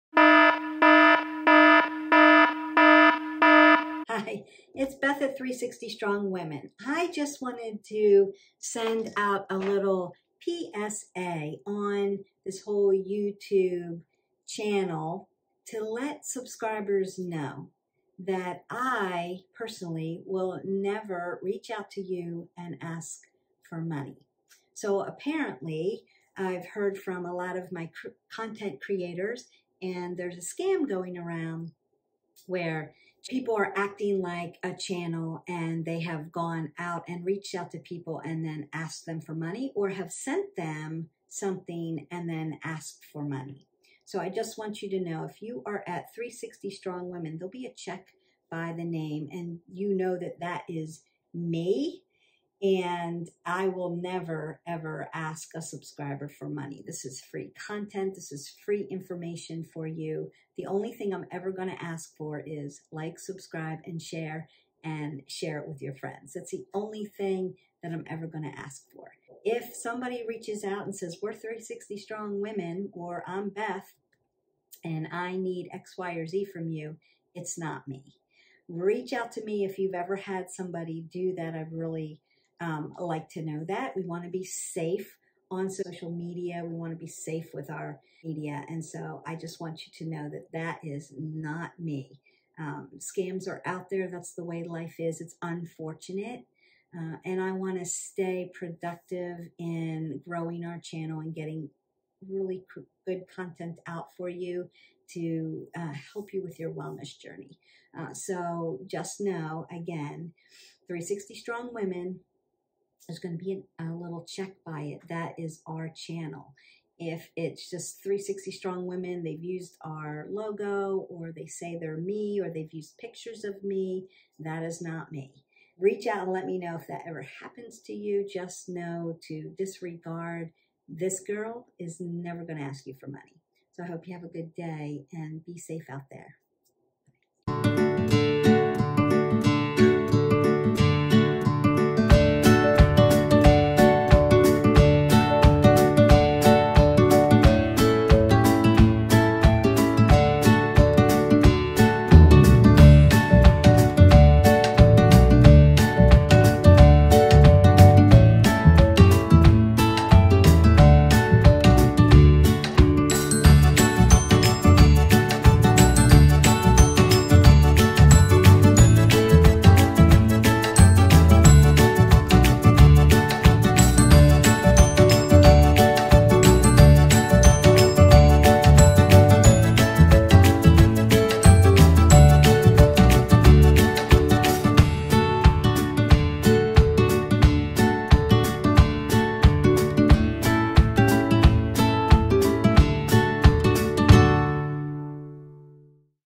NEW SCAM WARNING! Protect Your Money NOW!
A Publice Service Announcement Our channel will NEVER ask any subscriber for money! $. Our channels mission is to provide quality free content to our subscribers on all things wellness (fitness, movement, exercise, healthy eating, mindfulness, yoga, self care,slepp habits).  We want to let our subscribers and our future subscribers  know that we will never send you anything and ask for money or ask for money. We are aware that there are some scams out there that may be using YouTube profiles.  We also know that YOUTube is working diligently on this, which we appreciate.